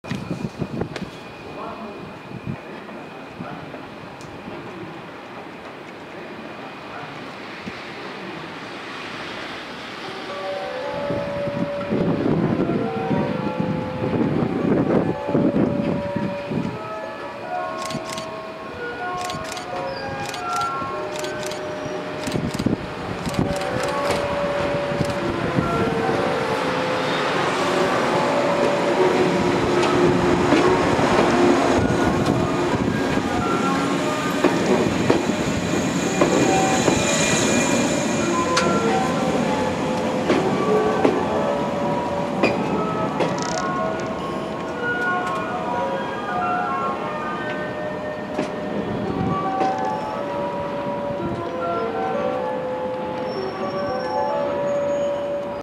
鳳駅入線する羽衣線103系HL101編成
HL101編成40Ｎ体質改善車による３両編成で運転されていた羽衣線